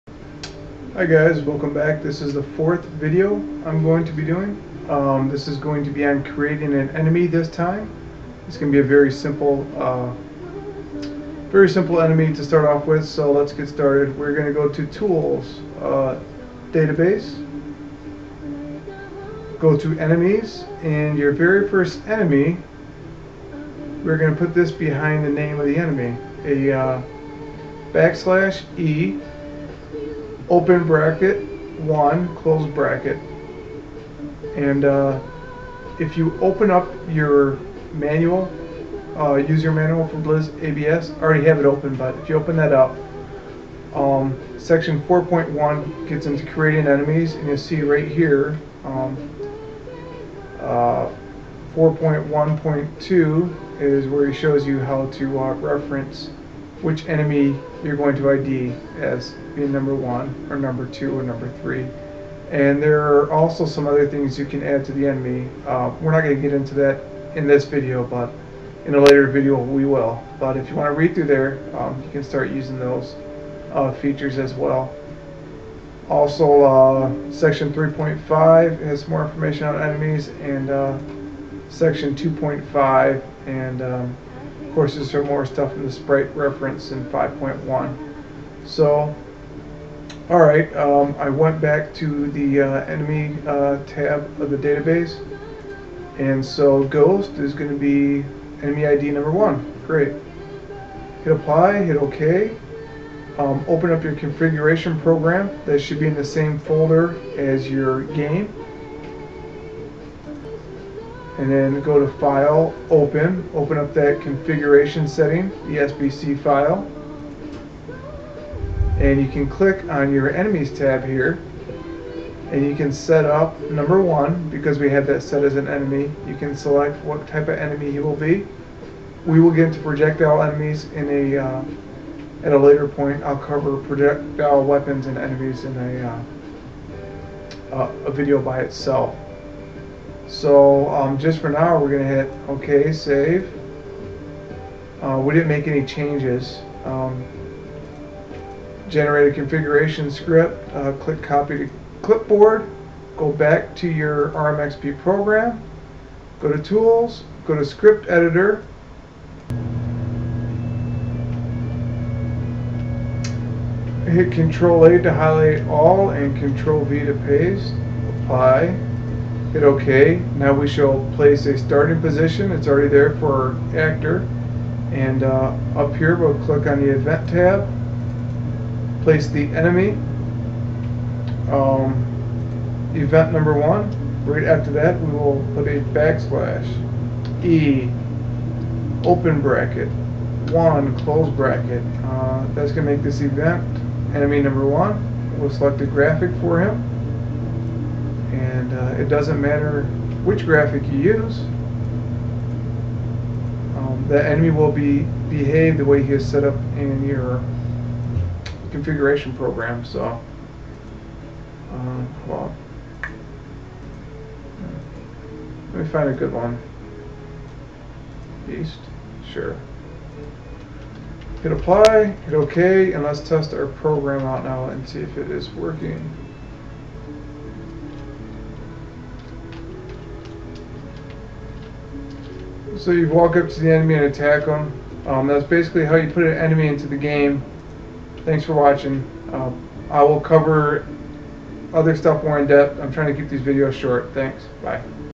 Blizz ABS Tutorial Part 4 (Create a very basic enemy)
A brief tutorial series covering Blizz ABS (script) for RPG Maker XP (rmxp).

This is an Action Battle System created by Blizzard at ChaosProject.com.